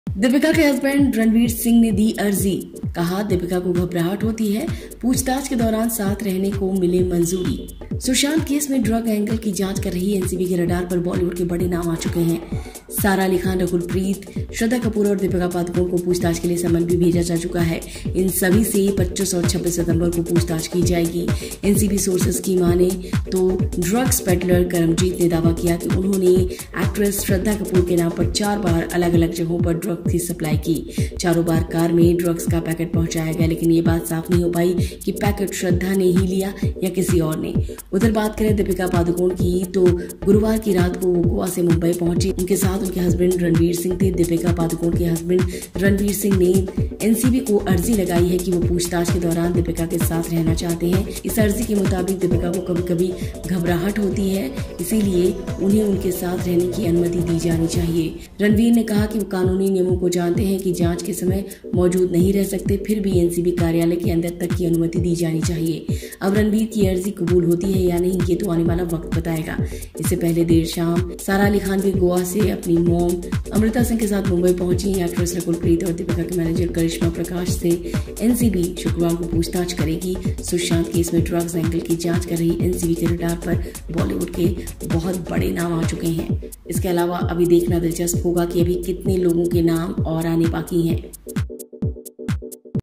Deepika के साथ Ranveer भी आखिर क्यों रहना चाहते हैं NCB की पूछताछ में? | FilmiBeat
Ranveer Singh to join Deepika Padukone in NCB interrogation.Watch video to know more.